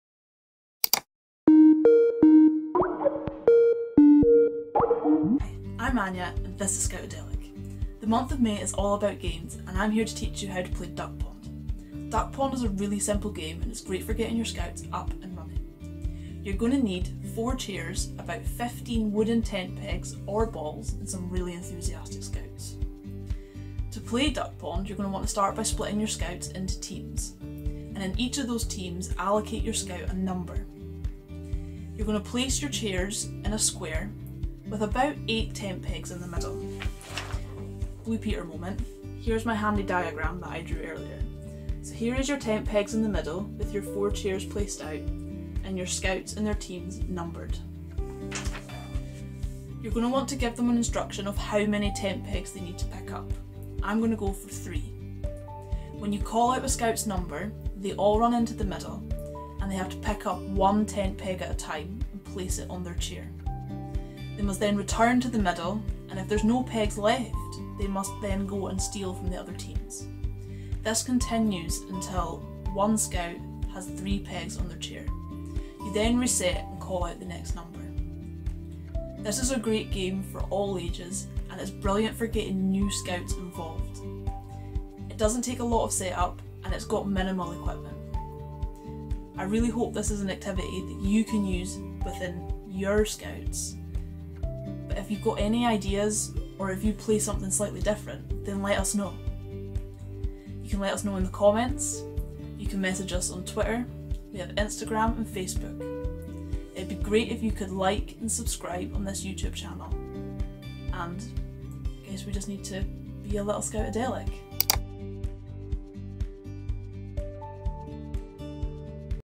HOW2 PLAY Duck Pond | SCOUTADELIC
Whilst Mike's off this week, Anja steps in to teach us how to play a simple game of Duck Pond, with minimal equipment needed. Can your Scouts get 3 wooden pegs before the others steal from their nests? Fun for all sections.